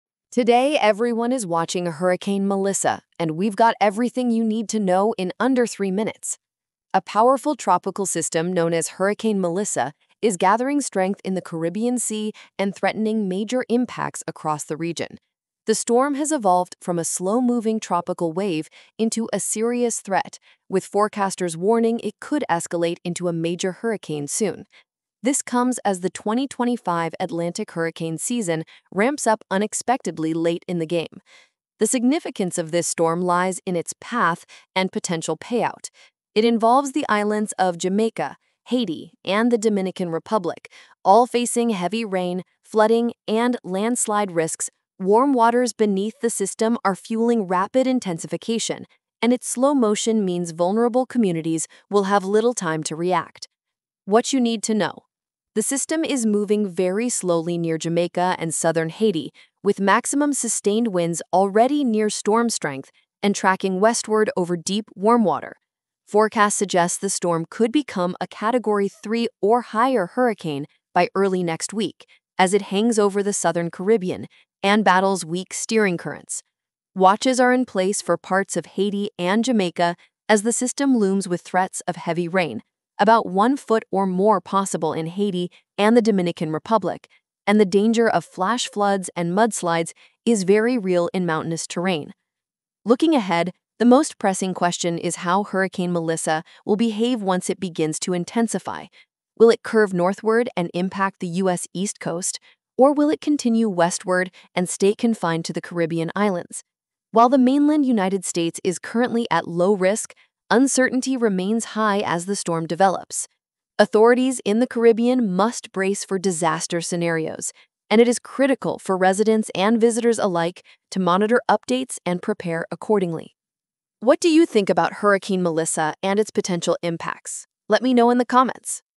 Hurricane Melissa’s Path: Caribbean on High Alert
Hurricane Melissa is rapidly intensifying in the Caribbean, and communities across Jamaica, Haiti, and the Dominican Republic are bracing for impact. In this video, we break down everything you need to know about this developing storm.

What we cover:
• How Hurricane Melissa formed and why it’s strengthening so quickly
• The areas at highest risk for flooding, landslides, and damaging winds
• Forecast models — will it stay in the Caribbean or turn toward the U.S.?
• Emergency preparations and what officials are urging residents to do now

This storm could become one of the most powerful systems of the season.